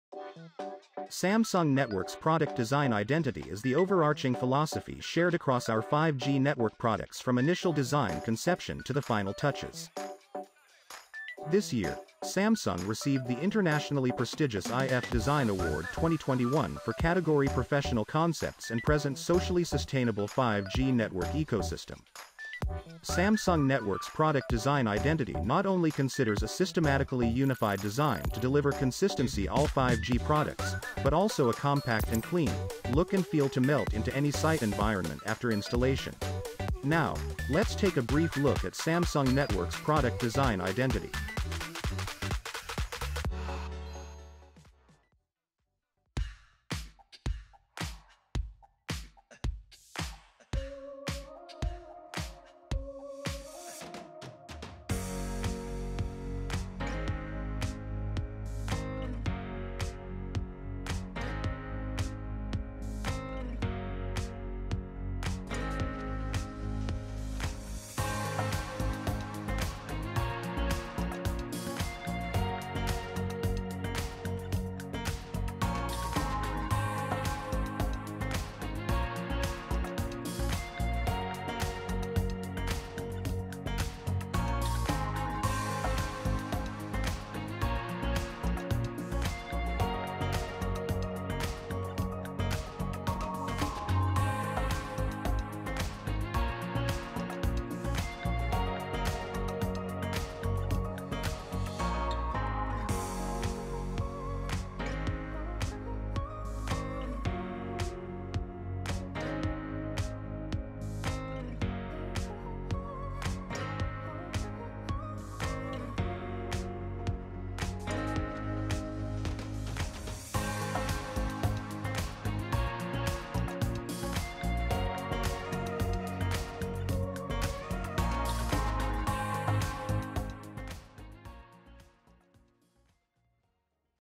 Samsung Networks Product Design Identity
Samsung received the internationally prestigious IF Design Award 2021 for our socially sustainable 5G network ecosystem under the 'professional concept' category.
Let's take a brief look at Samsung Networks Product Design Identity.